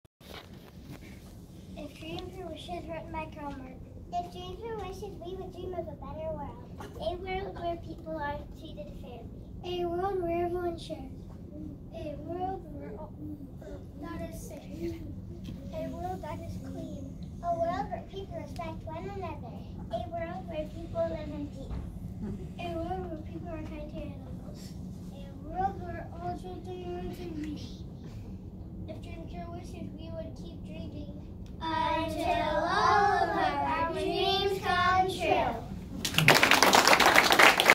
Piper's 1st Grade End-of-Year Show 1
The end of year pageant from Ms. Overly's 1st grade class with Piper.